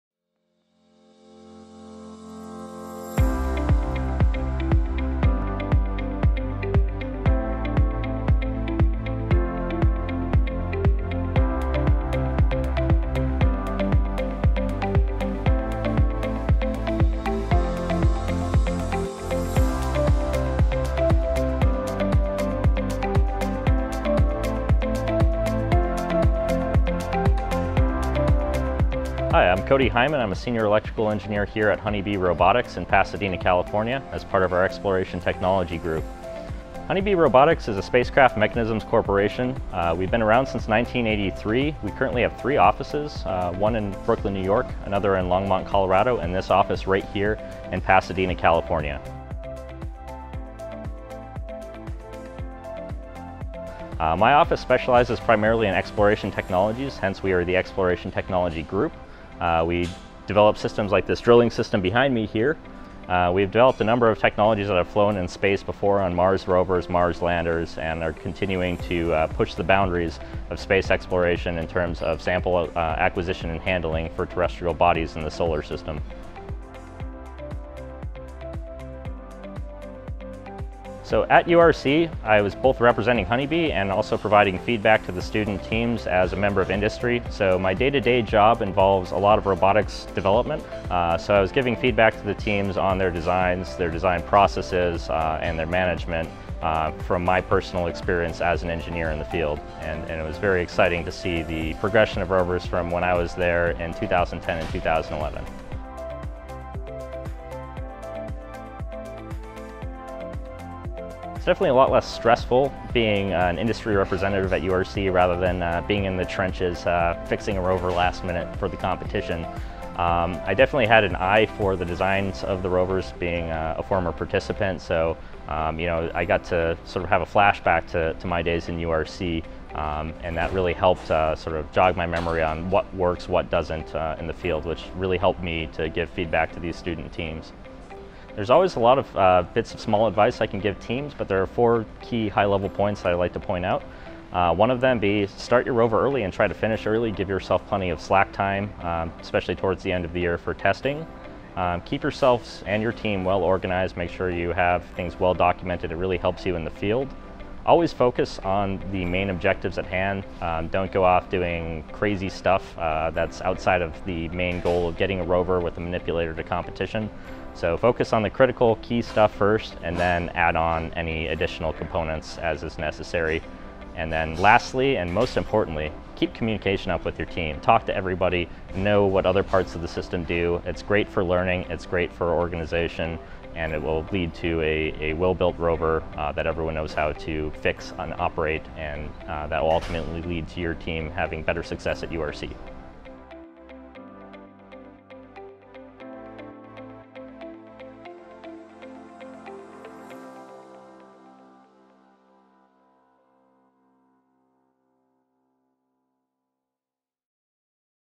An Engineer’s Advice for University Rover Challenge Teams, Featuring Honeybee Robotics’ Cody Hyman
Honeybee Robotics Senior Electrical Engineer Cody Hyman attended Protocase’s Industry Meet & Greet last year at URC 2017. Cody provided feedback to student teams at the event and gave design advice for future robotics development.

In this video, Cody highlights his experience returning to URC as an alumnus of the competition, and provides key high-level advice for teams designing Mars Rovers for URC 2018.

Protocase is proud to be a major sponsor of the University Rover Challenge!